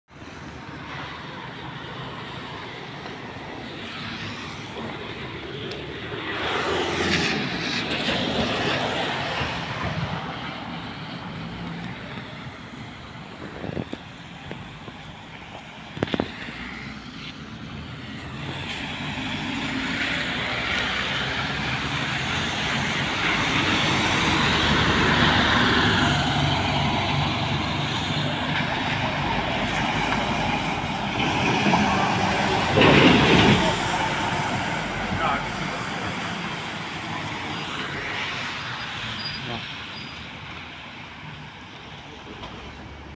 przejazd zdefektowanego MANa #2204 z źle chodzącą zepsutą skrzynią biegów ZF przy przystanku
Oksywie Dolne

+ przejazd tira